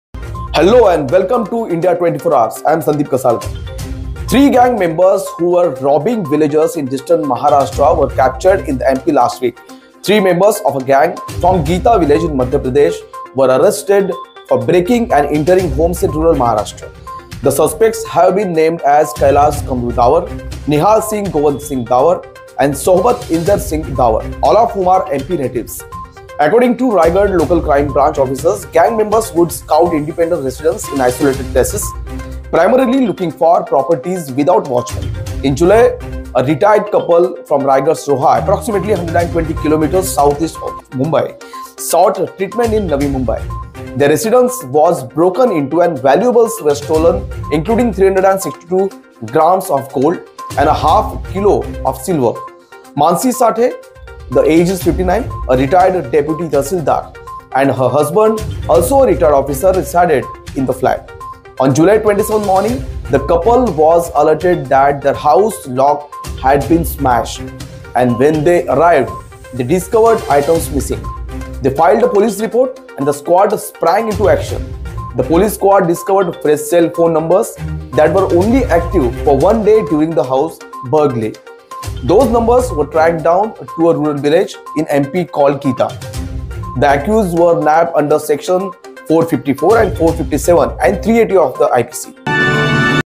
Three gang members who were robbing villagers in distant Maharashtra were captured in the MP
Last week, three members of a gang from Geta village in Madhya Pradesh were arrested for breaking and entering homes in rural Maharashtra. The suspects have been named as Kailas Kamru Dawar, 26, Nihal Singh Gowan Singh Dawar, 40, and Sohbat Inder Singh Dawar, 36, all of whom are MP natives.
According to Raigad Local Crime Branch officers, gang members would scout independent residences in isolated places, primarily looking for properties without watchmen.
In July, a retired couple from Raigad's Roha, approximately 120 kilometres southeast of Mumbai, sought treatment in Navi Mumbai. Their residence was broken into, and valuables were stolen, including 362 grammes of gold and half a kilo of silver. Mansi Sathe, 59, a retired deputy tahsildar (naib tahsildar), and her husband, also a retired officer, resided in the flat. On July 27 morning, the couple was alerted that their house lock had been smashed, and when they arrived, they discovered items missing. They filed a police report, and the squad sprang into action.
The police squad discovered fresh cell phone numbers that were only active for one day during the house burglary. Those numbers were tracked down to a rural village in MP called Geta.
The accused were nabbed under sections 454, 457 and 380 of the IPC.